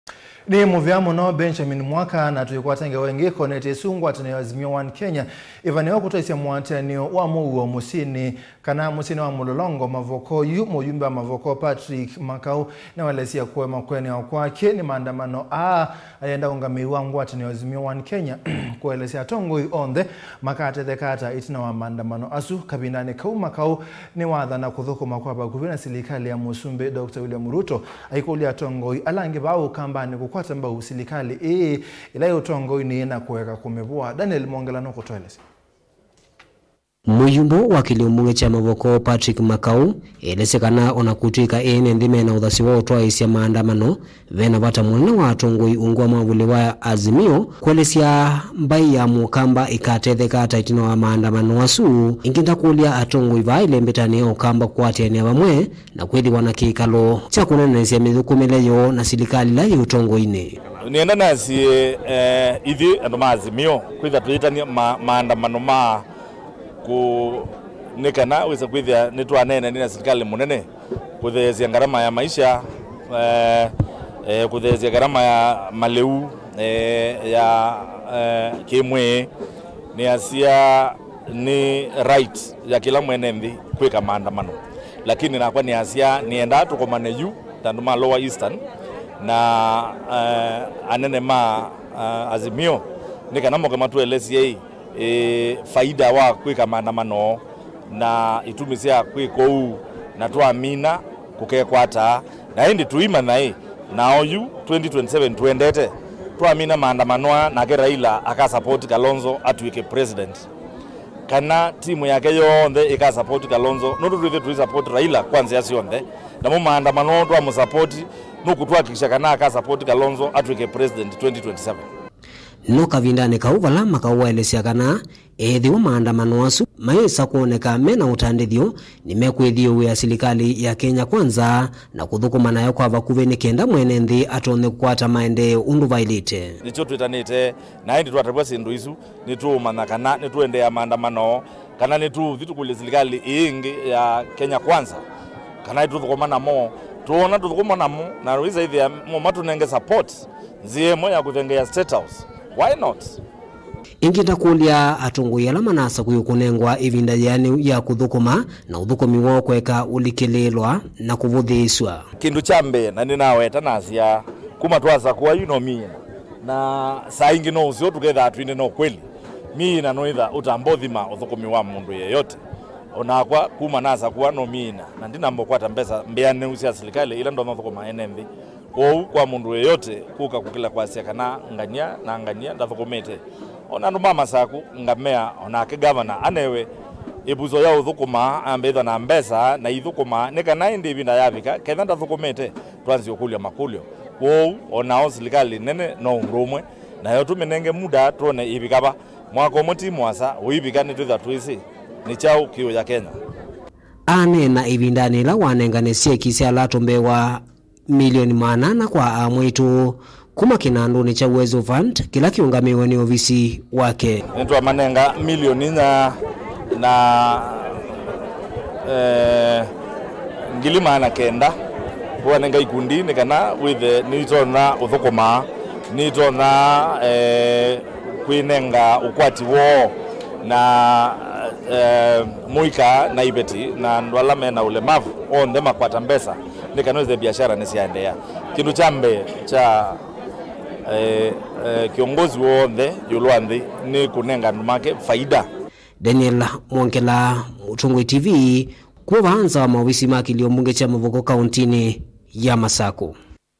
PATRICK MAKAU: MAANDAMANO MATEUTETHYA MUKAMBA MAINGWA VAITA! NITHUKUMA NA RUTO!
Ngwatanioni ya Azimio-One Kenya ivaniiwe kutwaaisya muatinio wa muuo musyini wa Mlolongo, Mavoko kauntini ya Masaku, yu muyumbe wa Mavoko Patrick Makau niwaelesya kuema kwianiwa kwake ni maandamano aa ayenda uungamii wa ngwatanio ii kuelesya atongoi onthe makatheka ata itina wa maandamano asu. Kavindani kau Makau niwathana kuthukuma kwa vakuvi na silikali ya president William Ruto, aikulya atongoi aangi vaa ukambani kukwata mbau silikali ila yi utongoini na kueka kumivua